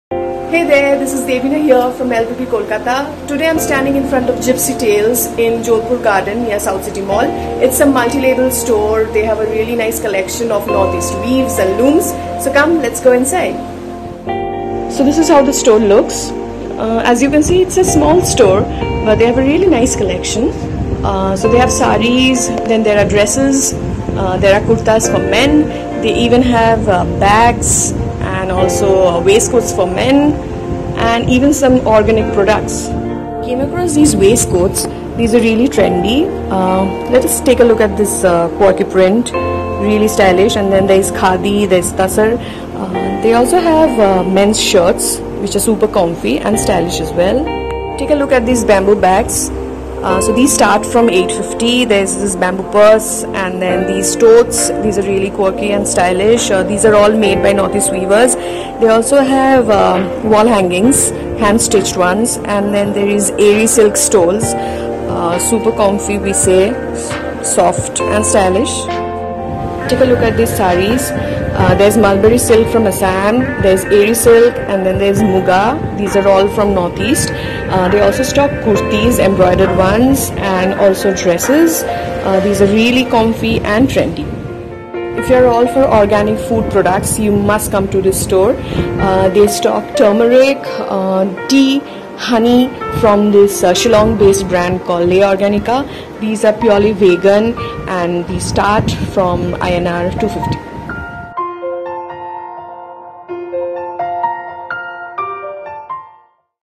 LBB kolkata presents The Gypsy Tales
Our vision is to connect Northeast India to the global market with a versatile product line .
       The Gypsy Tales is a nest for fresh homegrown weaves, hailing from Assam in the Northeast India. We took our first step in Kolkata, West Bengal(the gateway to East). We have labels from all across Northeastern hills including clothings(men and women), accessories (bamboo jewellery), organic herbs and spices. We also, have a wide range of Home furnishing, Bags, Artefacts, Stoles, Gamchas, Handkerchiefs. Our focus is to bring traditional attire and culture from the region into the mainstream narrative. The studio is not only a unit of stocking products but actually strives to celebrate the individual stories behind every design.